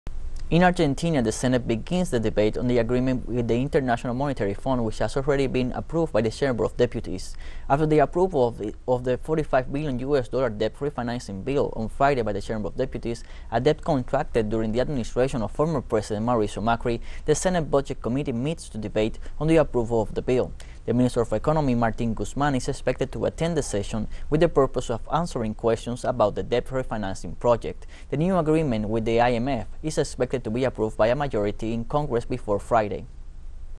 Argentine Congress debates debt with the IMF
On Friday, Argentine lawmakers from the Lower House approved a deal with the International Monetary Fund to restructure a debt of 45 billion dollars. teleSUR